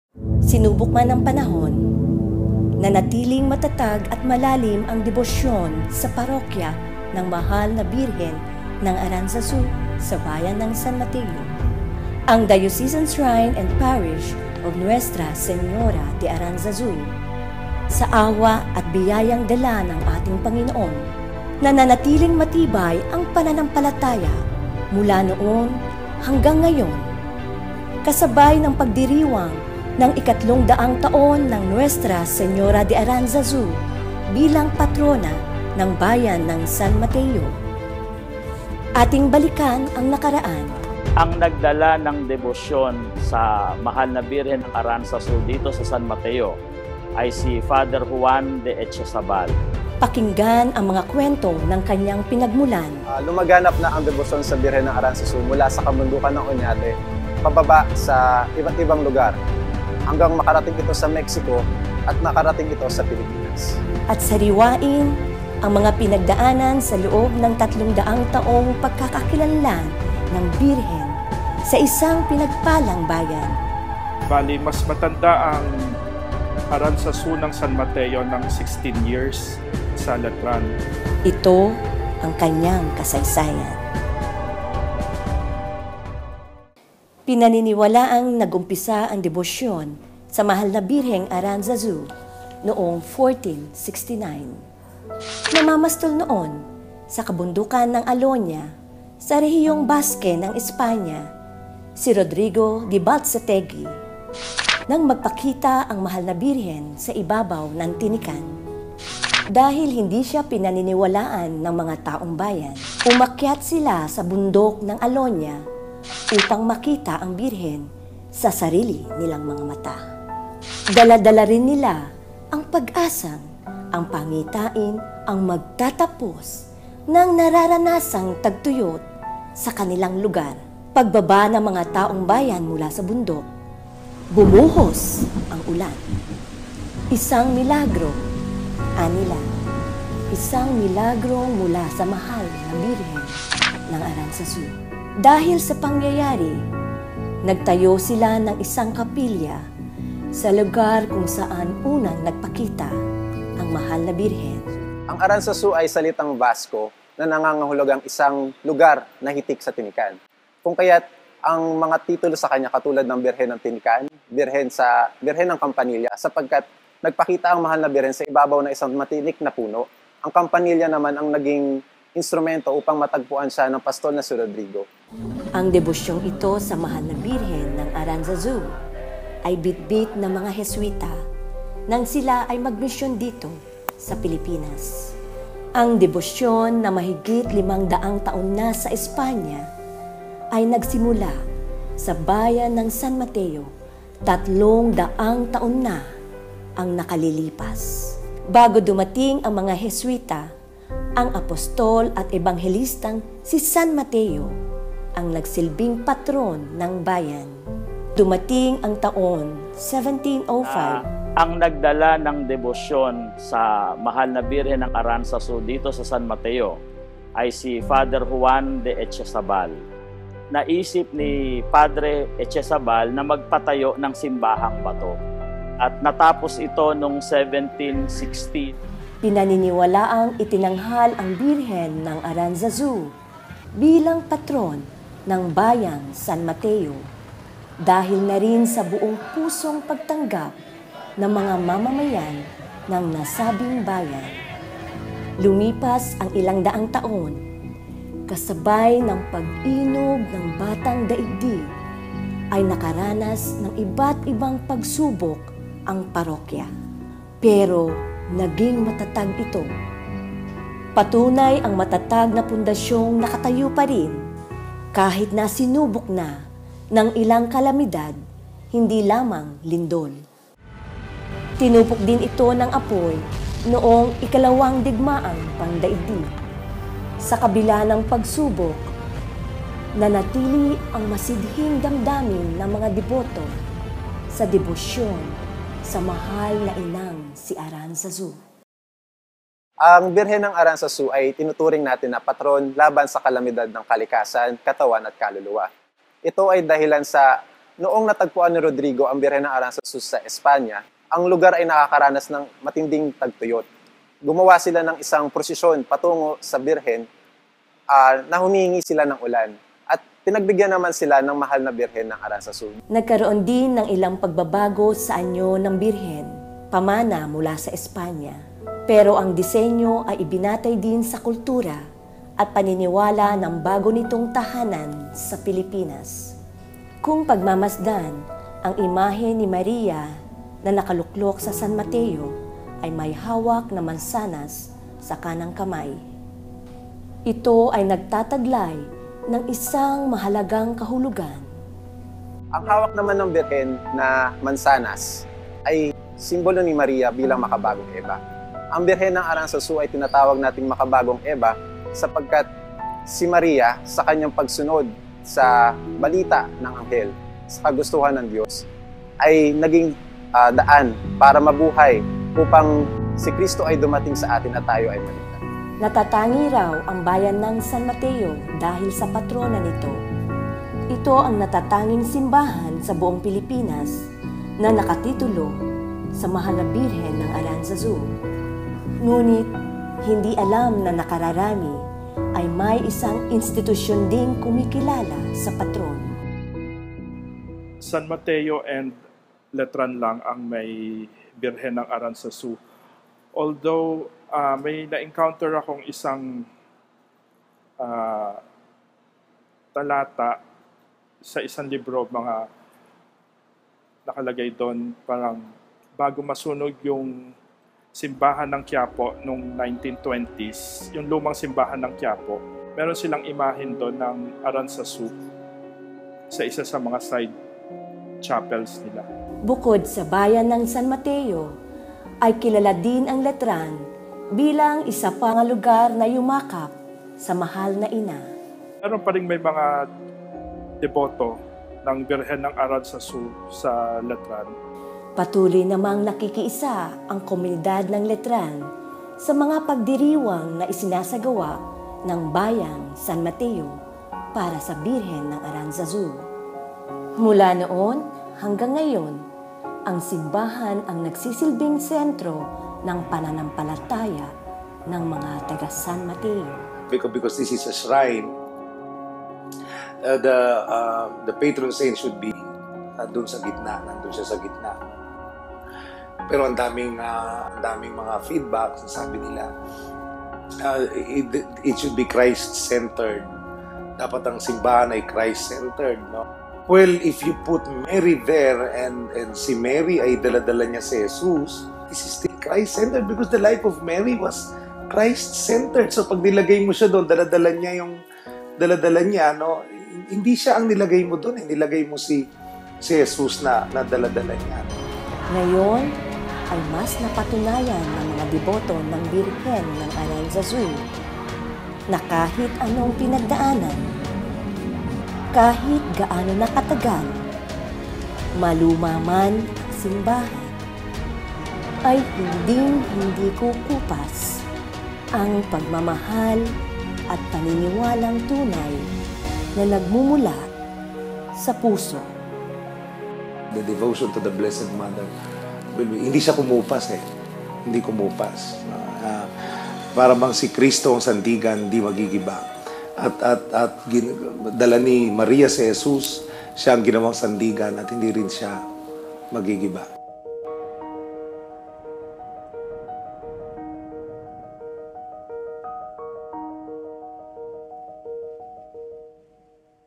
The History - Aranzazu@300
PART 2 of 3: THE HISTORY

Panoorin ang Pangalawa sa Tatlong Bahagi ng Documentary Film Series tungkol sa ika-300 Taon Debosyon sa ating Patrona, Nuestra Señora de Aranzazu. Magdiwang ang Bayang Pinagpala.